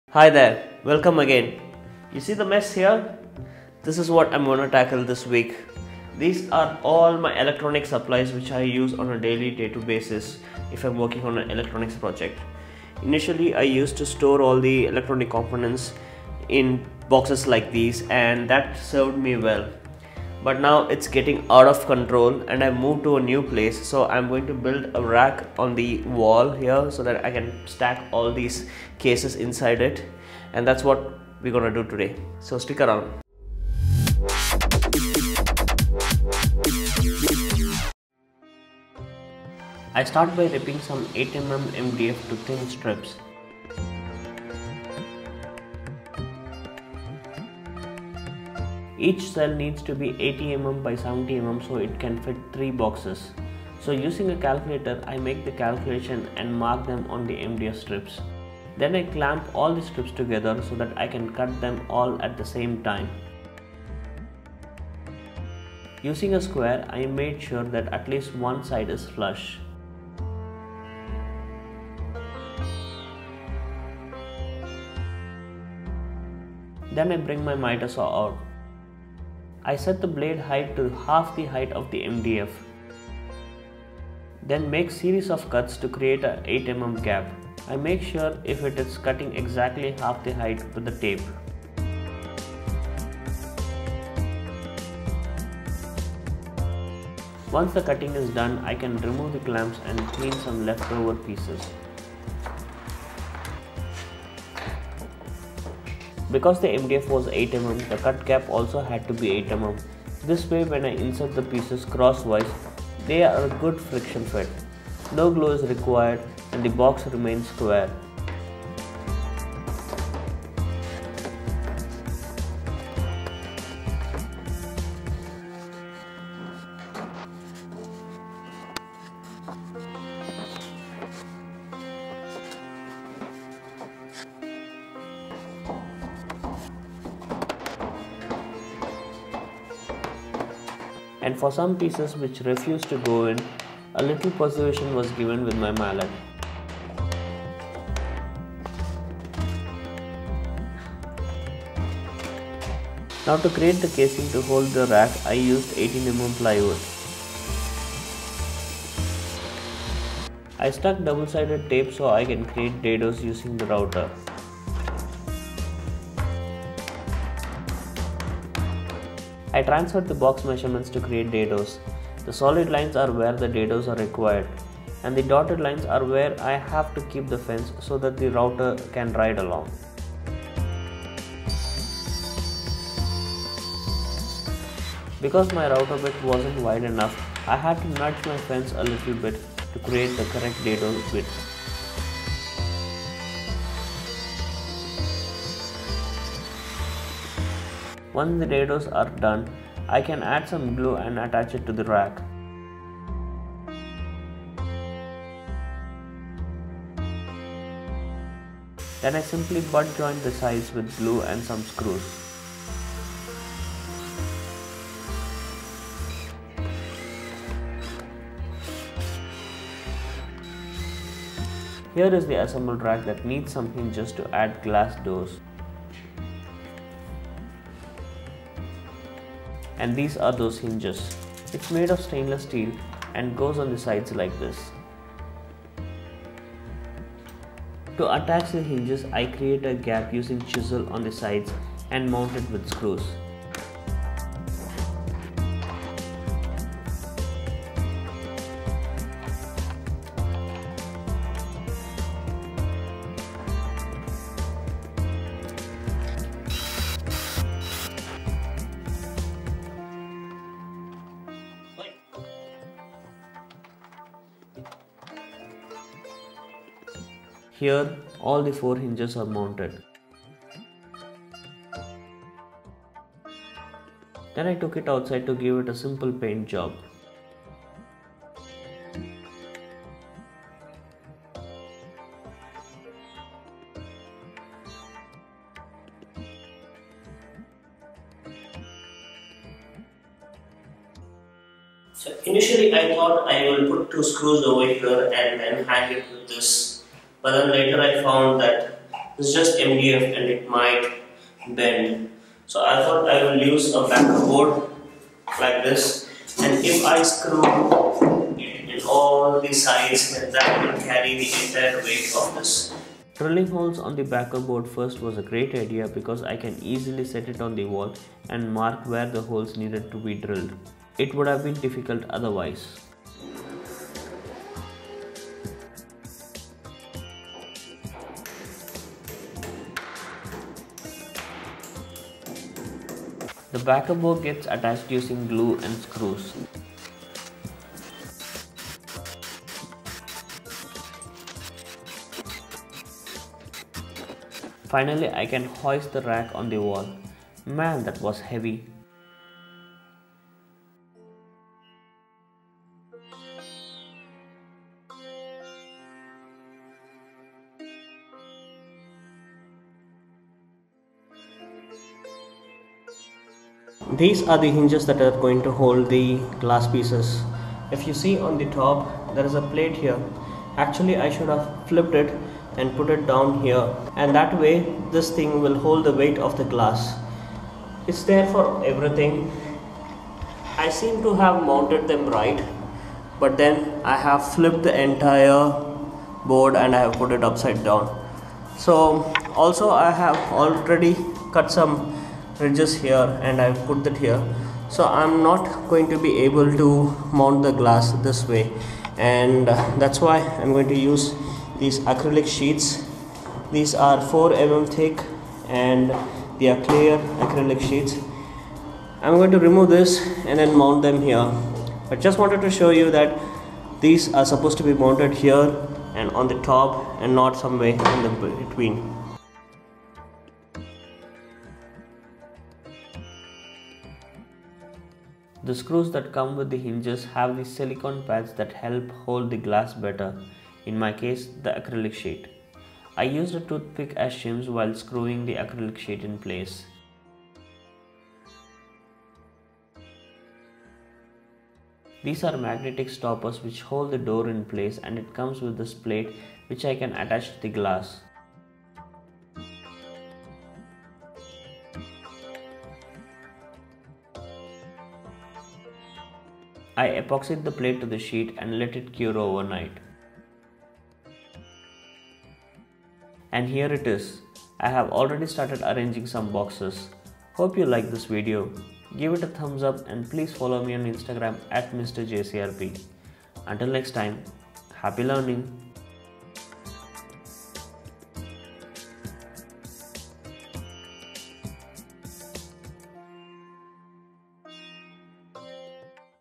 MDF Electronics Rack with friction fit
Electronics components are usually very small and don't take up much space. But is very necessary that they are kept sorted and easy to reach. To tackle this issue I built this electronics rack with a simple friction fit design.

MORE PROJECTS TUTORIALS and POSTS